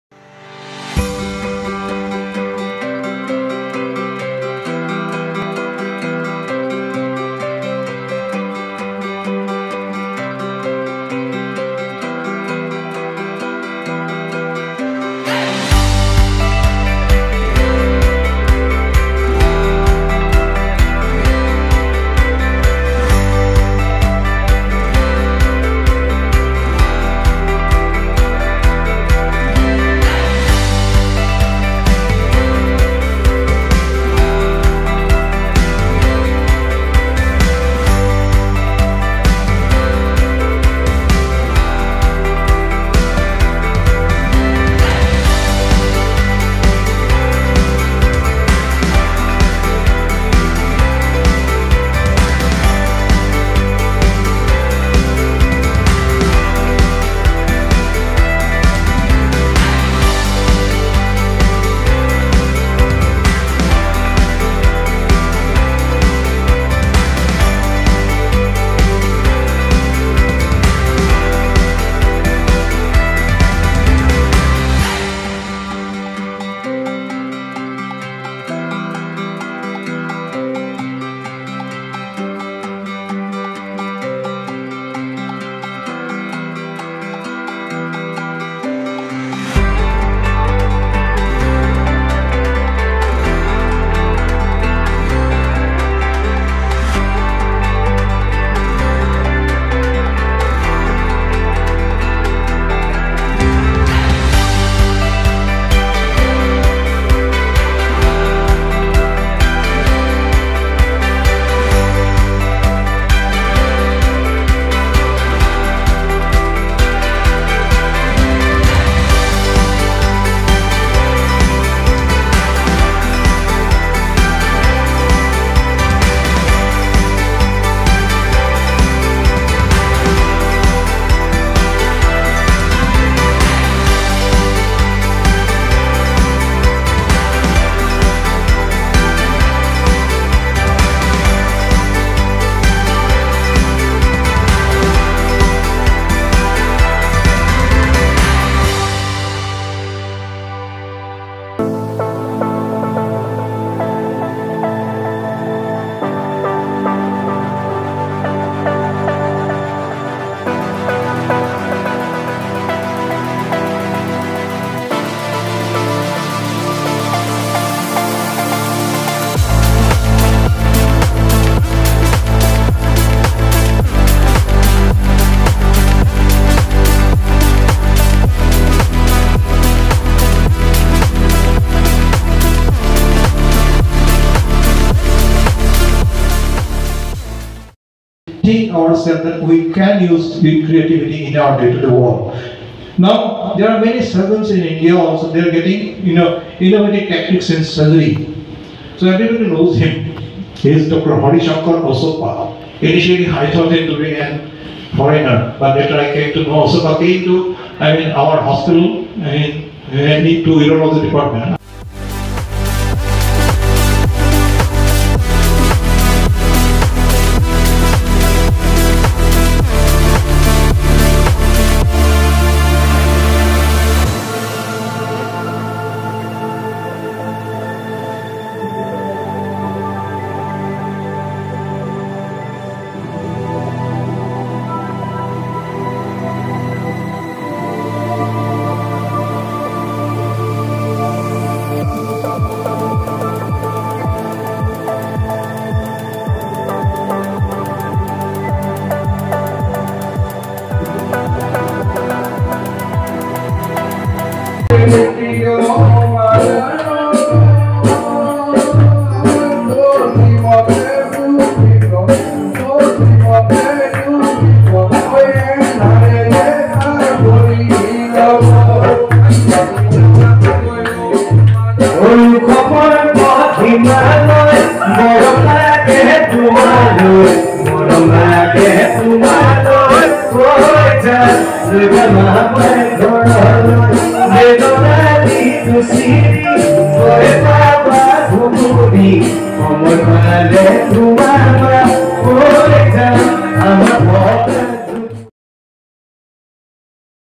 A Glimpse of ASACON 2022 | ASA conference in Nagaon
A Glimpse of ASACON 2022//ASA conference in Nagaon held at refresko on 22nd & 23rd October 2022. The Association of Surgeons of Assam is a chapter of the Association of Surgeons of India (ASI).

asacon 2022
2022 asacon
asacon nagaon
surgeons conference of Assam
refresko nagaon
nagaon refresko

Video: Diganta Talukdar
NOTE A PROMOTIONAL VIDEO